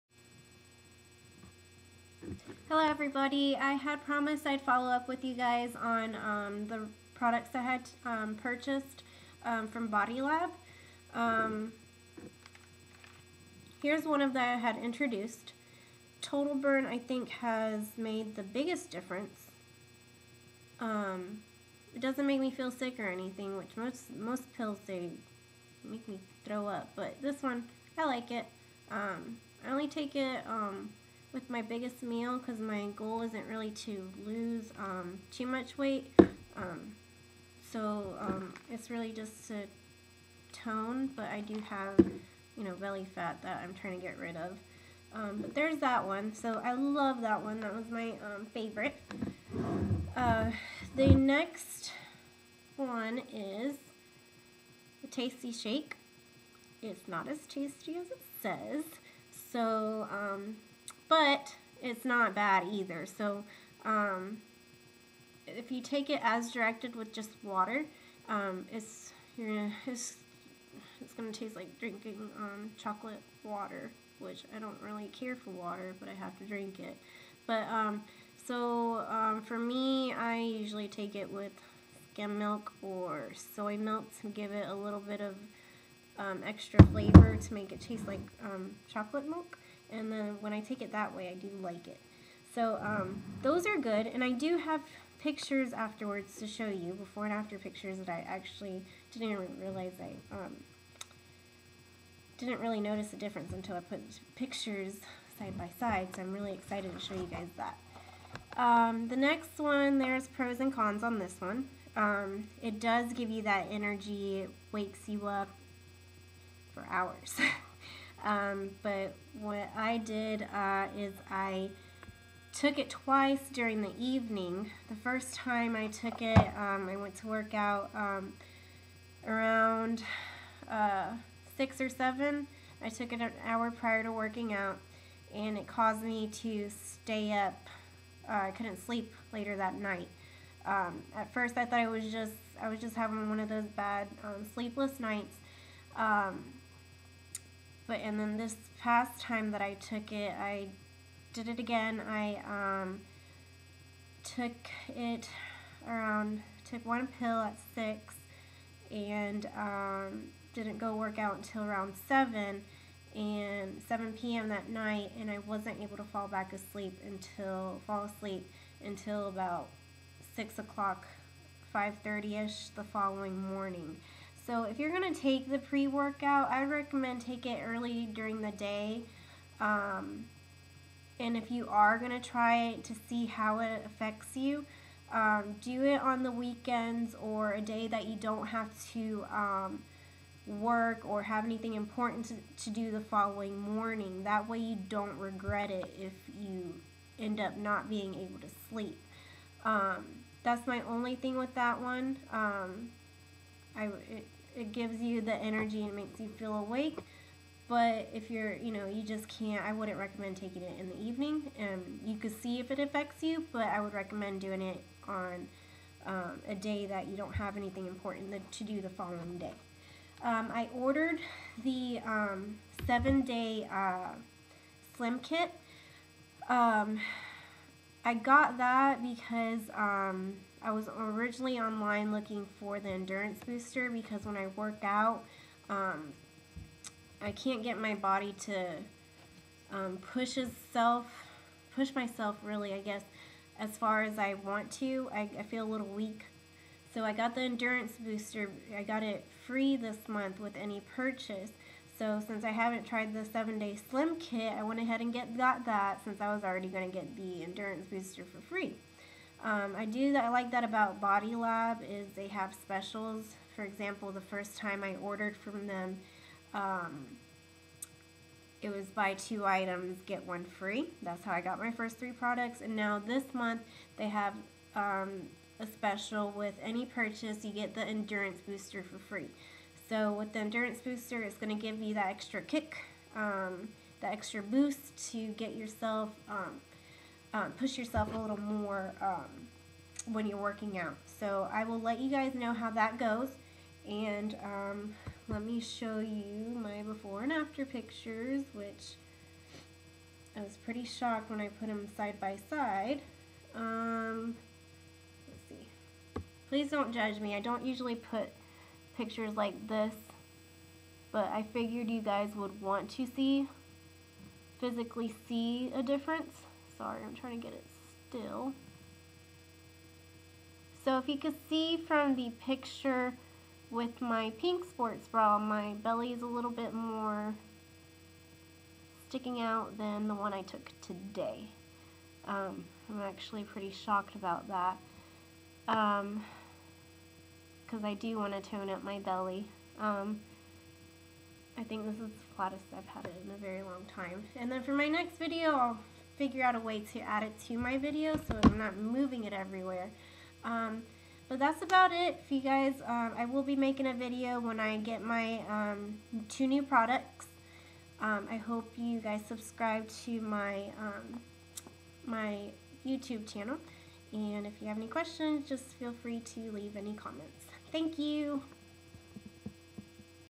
Body Lab jlo product review with before and after pics
Here is my 30 day product review with before and after pictures! Overall I love the products thus far and happy to see results!! Hope this helps y'all!! Products are from bodylab.com